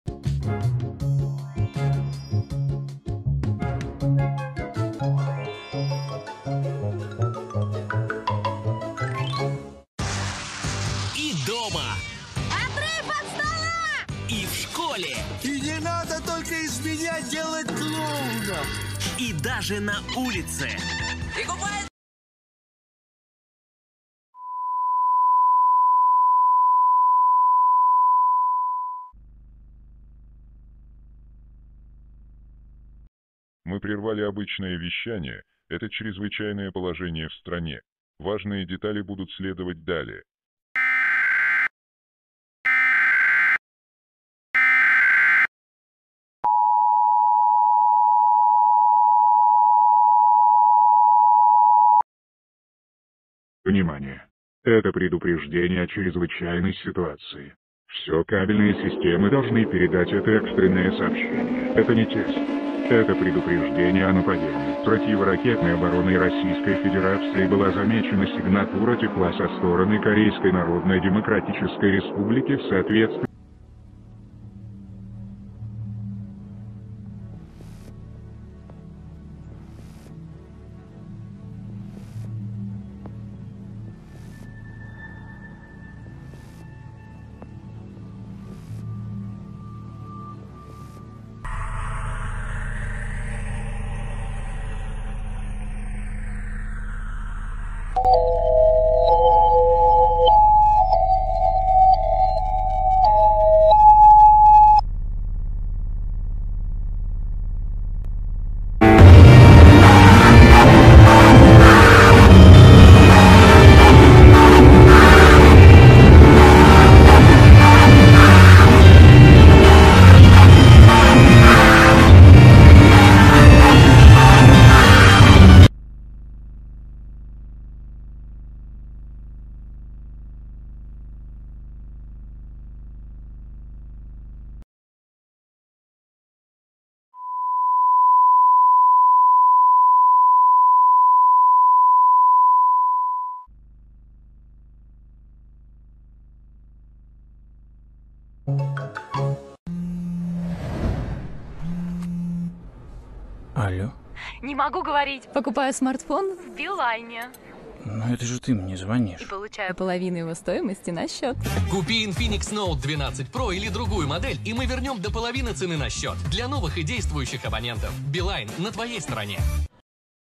[EAS/Analog Horror] Перехват телевещания "Карусель" [23.02.2020]
Этот сценарий взлома ТВ является фан-мейдом и фейком.
This TV hacking scenario is a fan-made video. Don't take everything shown in the video seriously!